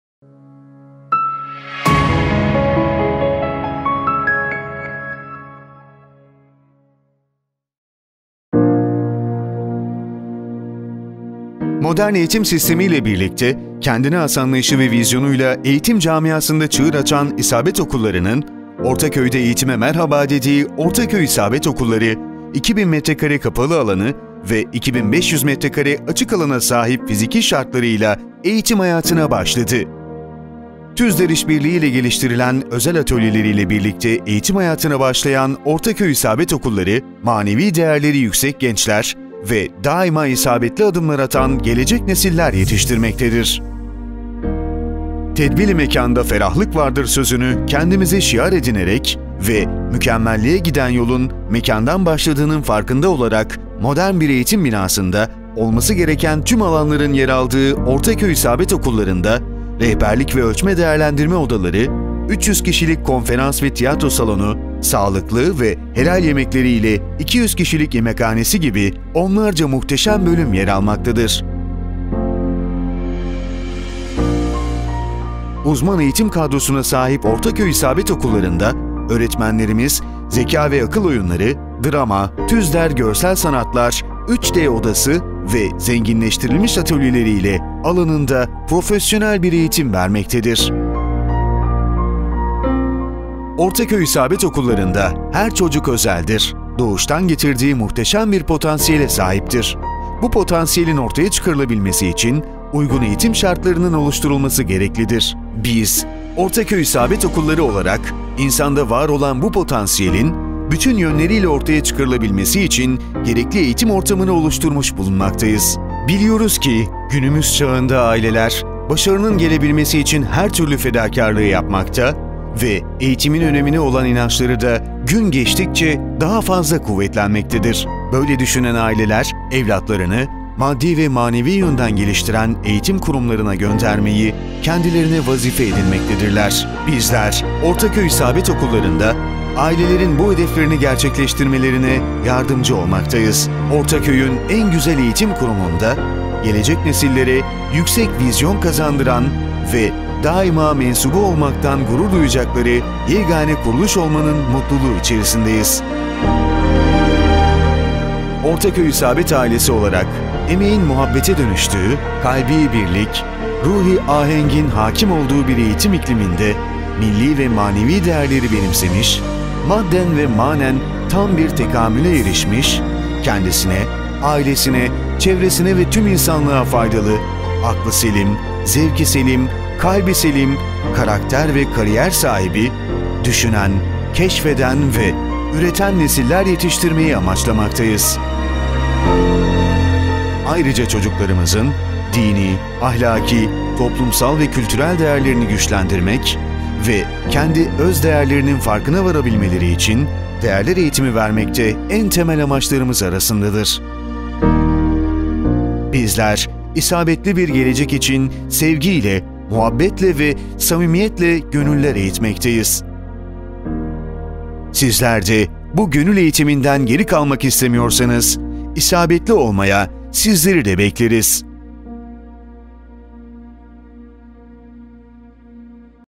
İsabet Okulları Aksaray Ortaköy Anaokulu - Tanıtım Filmi
Aksaray - Ortaköy'de hizmet vermeye başlayan İsabet Okulları Ana Okulu için hazırlamış olduğumuz tanıtım filmi.

Kendilerine başarılar diliyoruz.